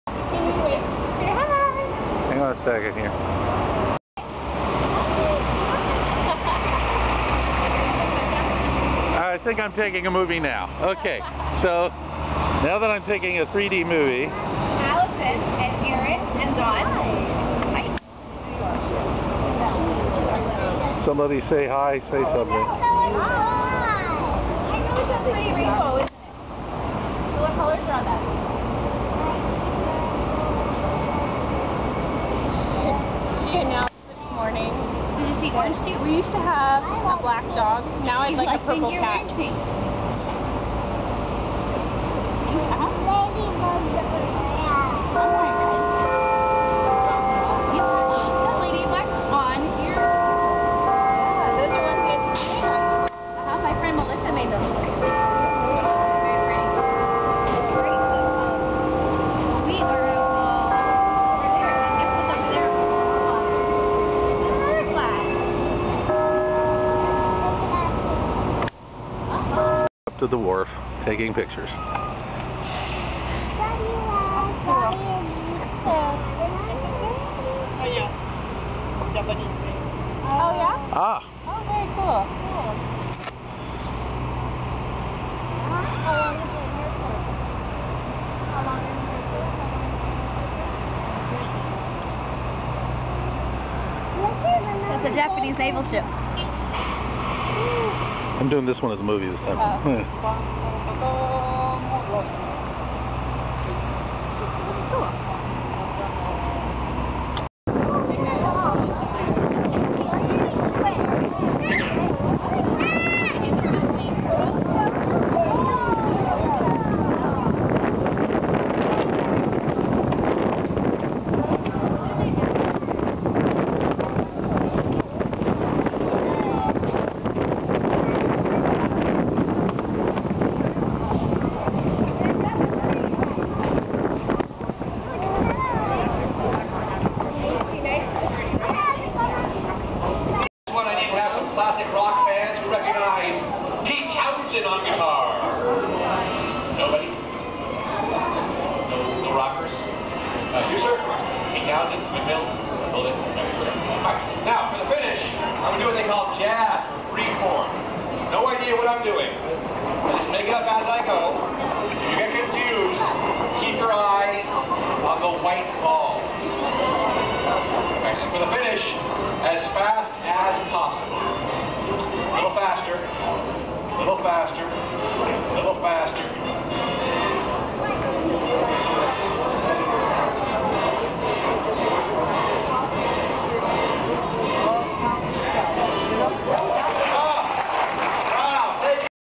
3d embarcadero SF 2011
San Francisco vacation June 2011 Embarcadero, pier, seals, juggler, (Gary), Tina, Dawn, Allison, Erin